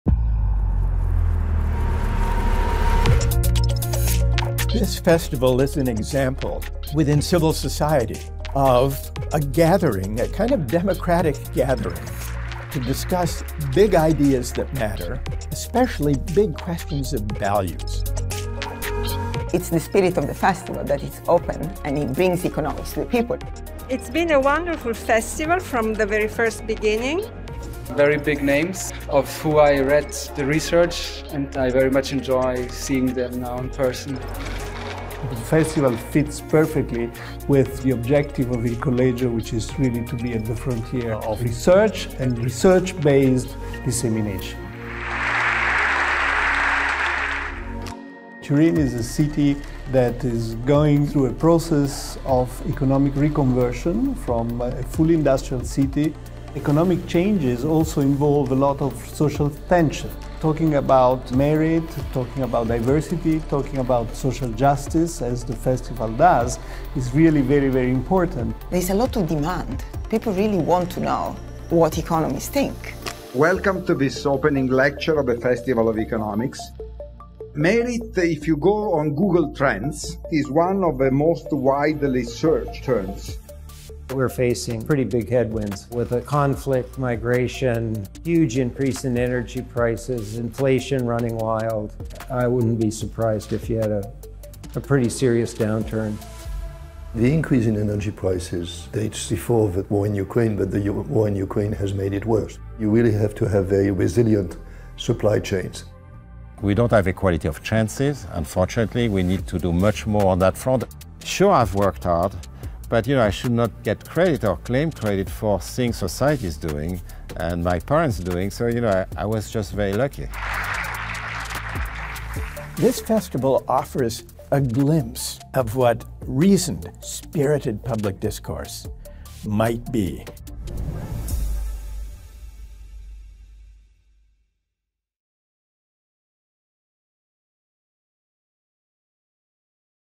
Festival Internazionale dell'Economia 2022
Scientific research, informal but rigorous dissemination, pluralism of ideas: these are the
components of International Festival of Economics (FIE), directed by Tito Boeri, designed and
conceived by Editori Laterza and organized with the Torino Local Committee (TOLC).
The event takes place in Turin from a public initiative led by TOLC, which brings together the
Piedmont Region, City of Turin, Fondazione Compagnia di San Paolo, Fondazione CRT, the University
and Politecnico of Turin, Turin Chamber of Commerce, Industry, Crafts and Agriculture, Unioncamere
Piemonte, Turin Industrial Union, coordinated by the Fondazione Collegio Carlo Alberto.
The theme of the 2022 edition was “Merit, Diversity, Social Justice”. From June 1-4, 2023 the second
edition will address the issue of “Rethinking Globalization”.